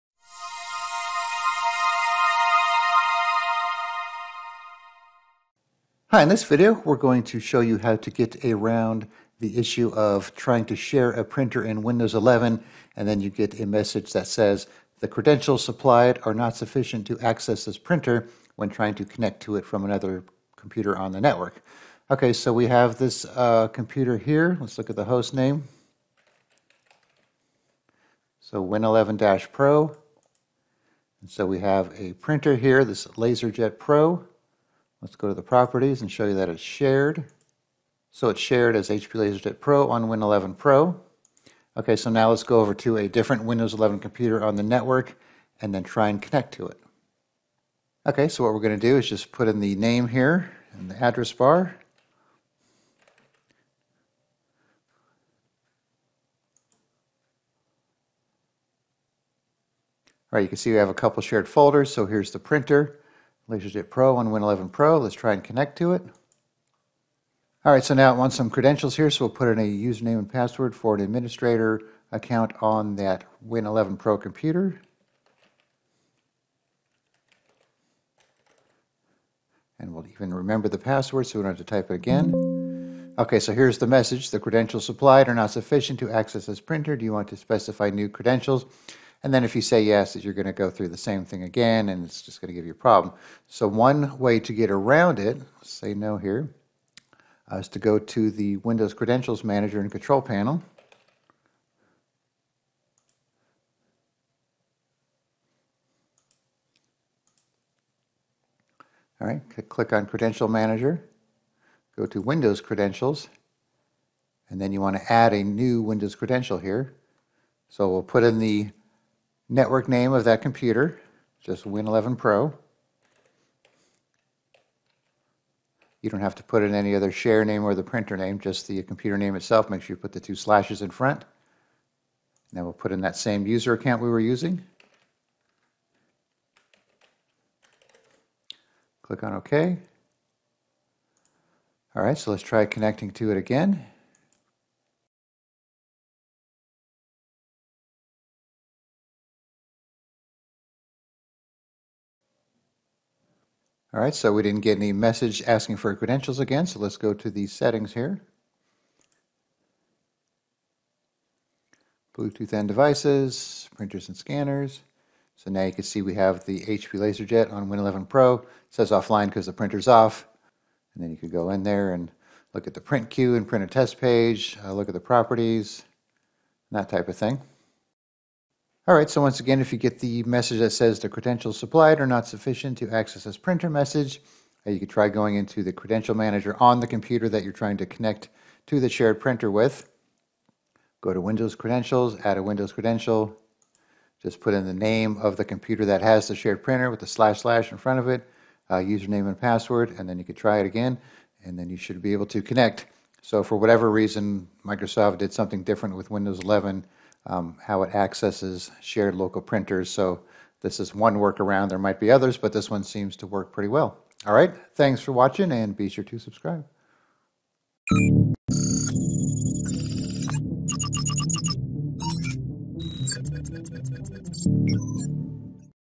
Fix the Windows 11 The Credentials Supplied are not Sufficient to Access This Printer Error Message
If you are trying to connect to a shared printer using Windows 11, you might receive an error message saying "The credentials supplied are not sufficient to access this printer" after entering the credentials for a user on the computer sharing the printer, there is one method you can try to get around the error and successfully install the networked printer. This process involves adding a new Windows credential in the Credential Manager tool in Control Panel.